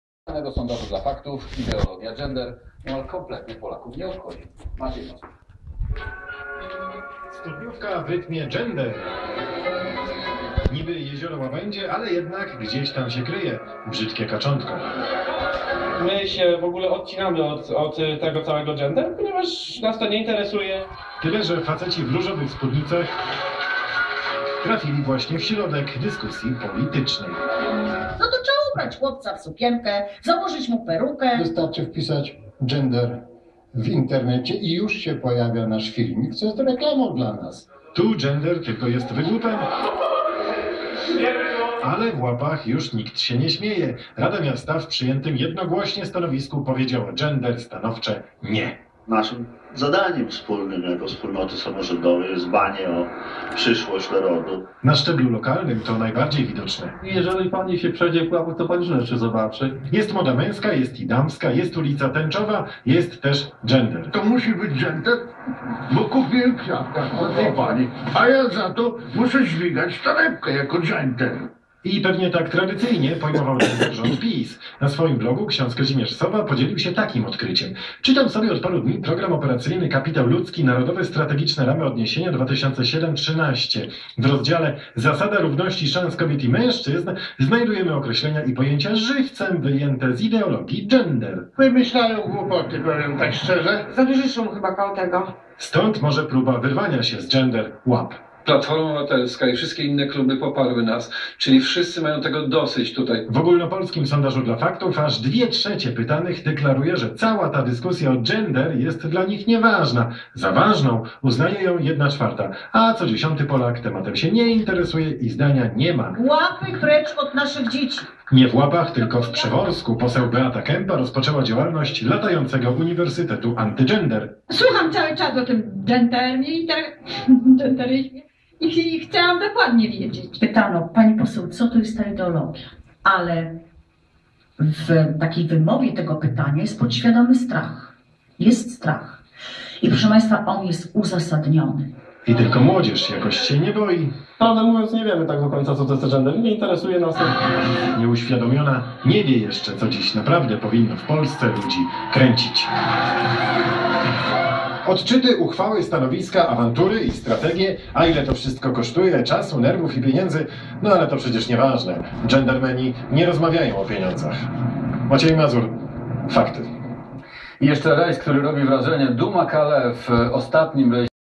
GENDER NA WIOSCE!!!
WQ..ŁO MNIE DO ŁEZ!!!
Nie zajmuje się polityką ale gdy zobaczyłem w TVN byłego burmistrza-posła, który rozpieprzył w mojej wiosce dwa największe i jedyne tak duże miejsca pracy, który pieprz-zajmuje się gender to trafił mnie szlak.
Pytam się - GŁUPOTO!!! GDZIE SĄ TWOJE GRANICE???
We wiosce bieda aż piszczy i pracy nie ma a idiota za pieniądze podatników zajmuje się TAKIMI IDIOTYZMAMI!!!
Panie Czepa - jest pan IDIOTĄ!!! :(